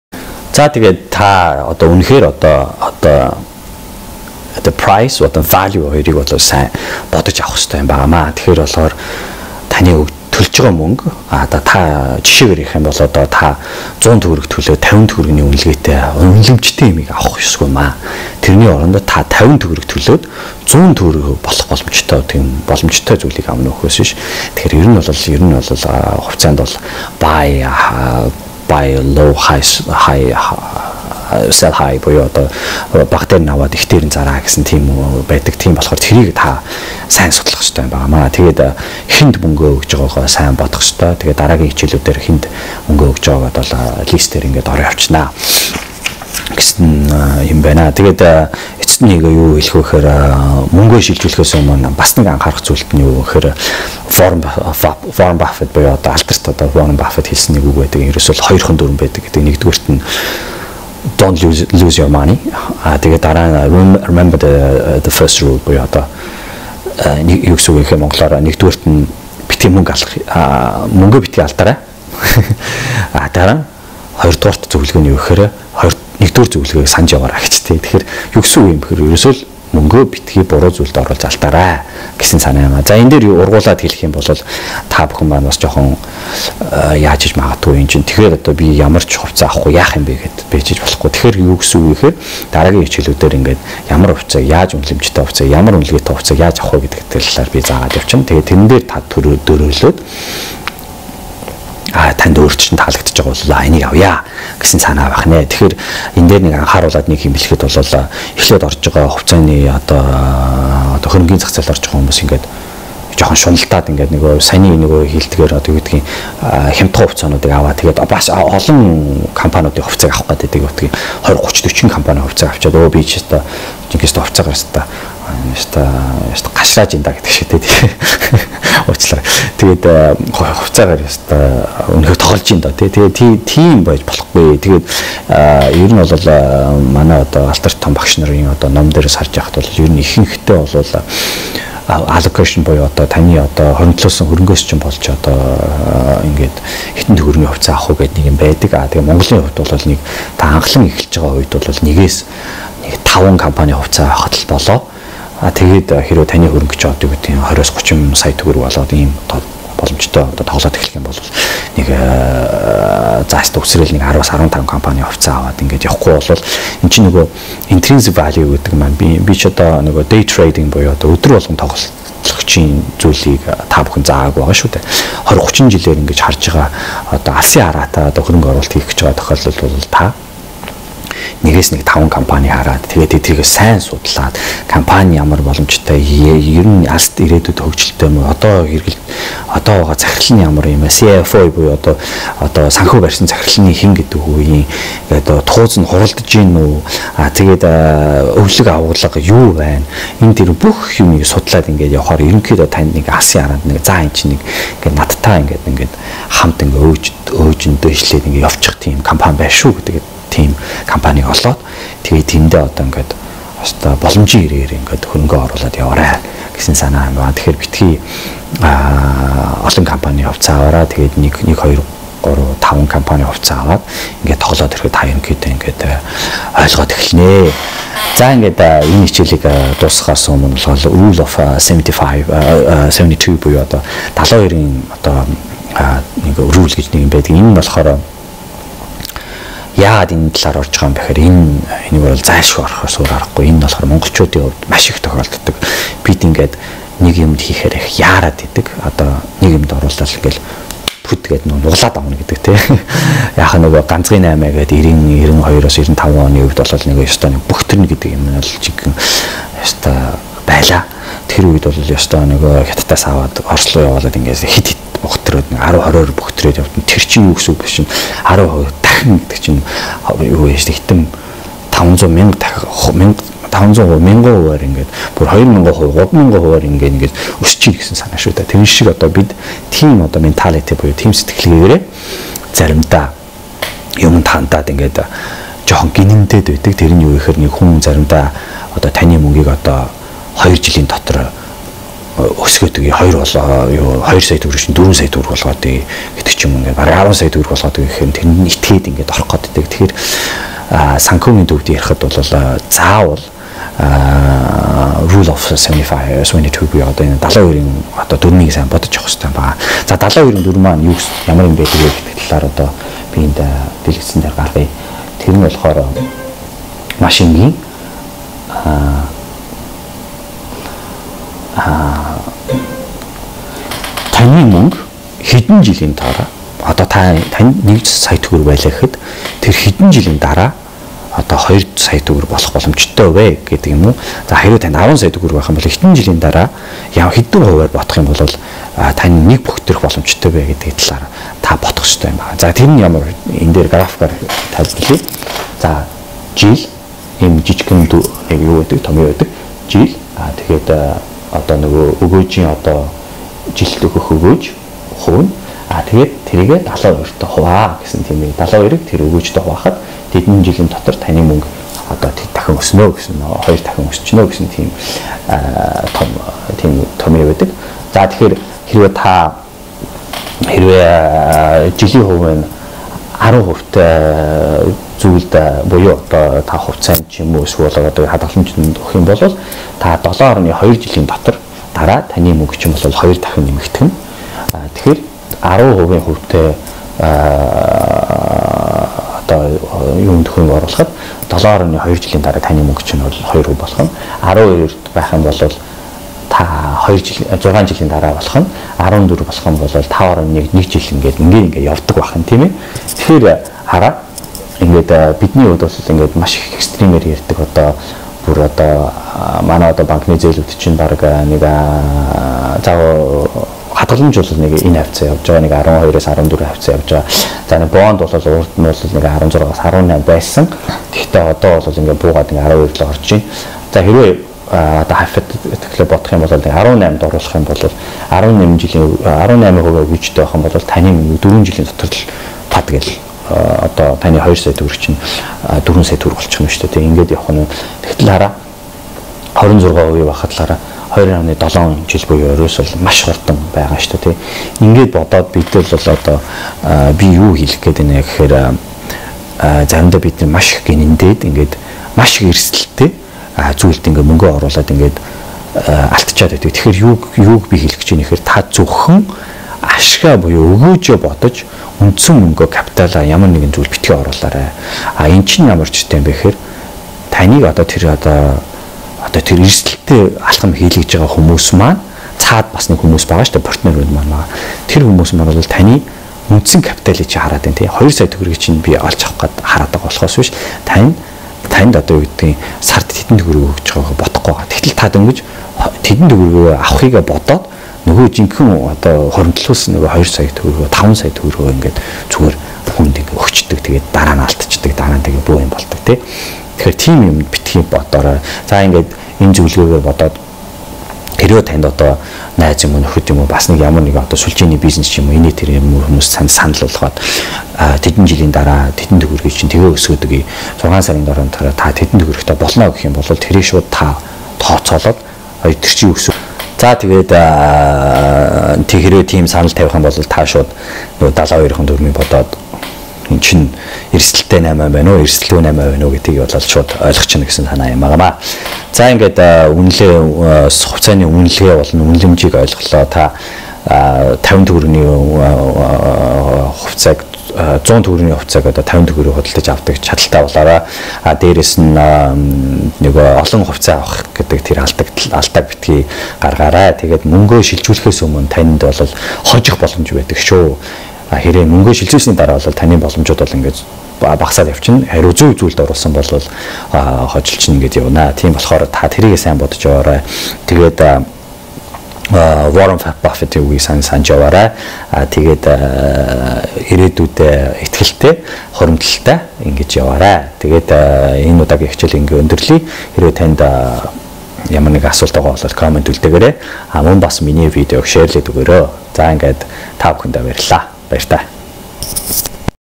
Цуврал 07: 2-р 72 дүрэм гэж юу вэ? Ашиг биш эрсдэлийг бодож хөрөнгө оруулax
Цуврал 07: 2-р 72 дүрэм гэж юу вэ? Ашиг биш эрсдэлийг бодож хөрөнгө оруулax сэдвээр үргэлжилж байна.

Та бүхэн хүлээнэ авч үзнэ үү. Мөн коммент бичих шэйрлээд өгөөрэй! Баярлалаа.